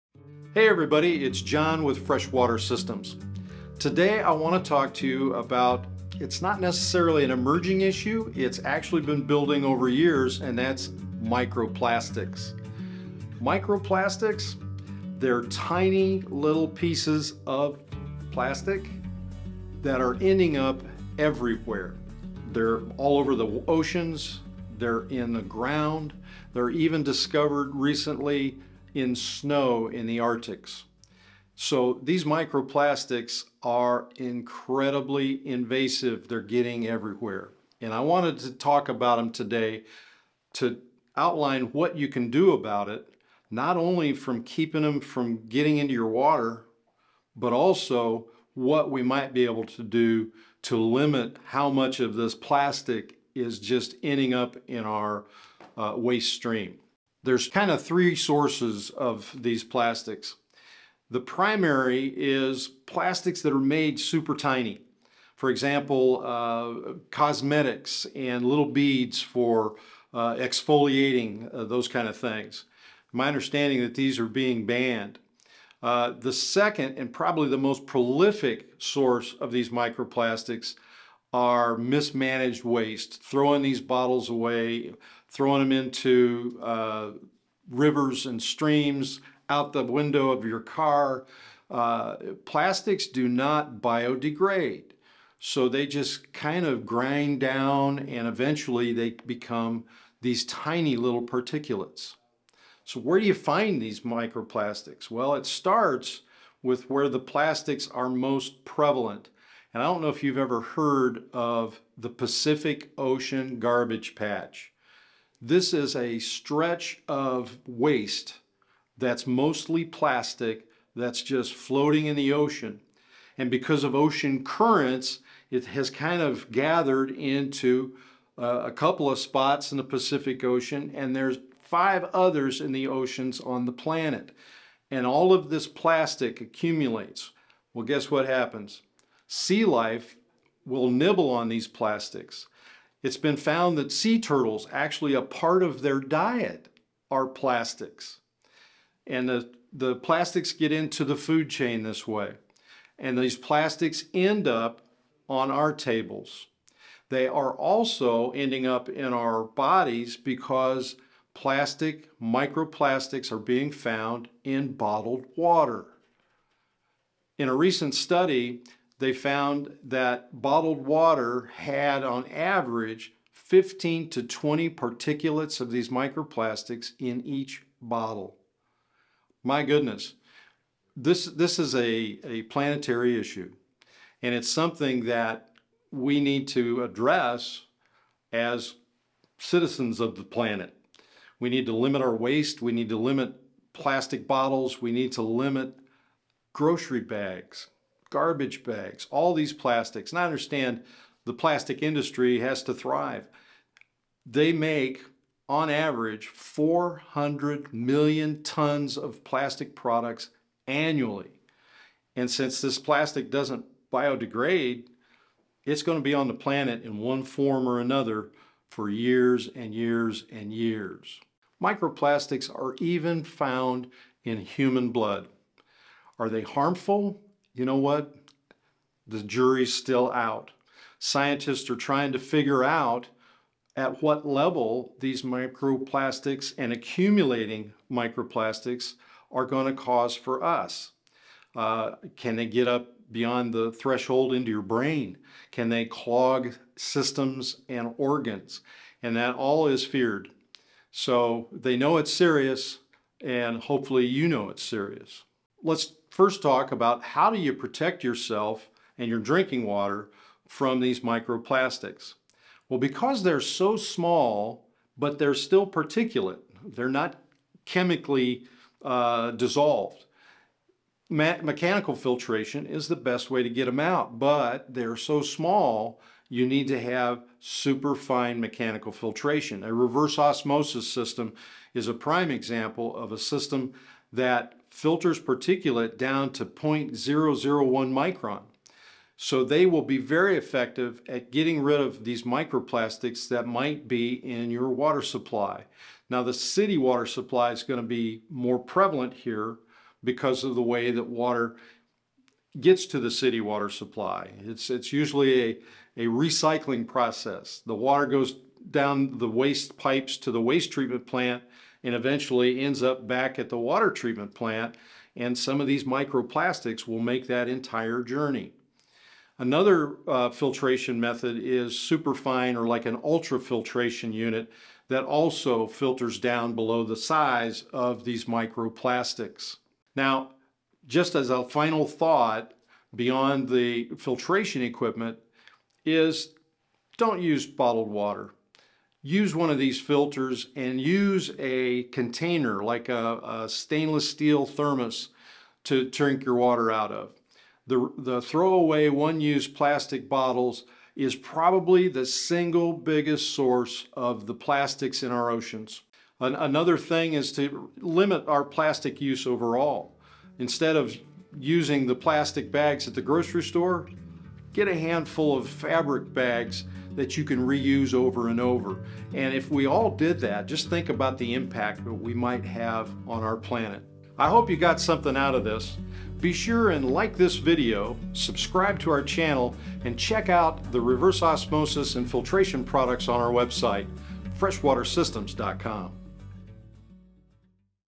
Are Microplastics Bad for You? The Truth about Microplastics and How to Remove them from Water
Microplastics are big in the news right now because of their impact on human health and the environment. They seem to be everywhere -- what can be done about microplastics in your water? Master Water Specialist John Woodard from Fresh Water Systems explains the problem of plastic and what you can do to limit the release of microplastics in our environment and in your home's water system.

💦 Shop our new NEO brand Reverse Osmosis System, made more efficient with a permeate pump!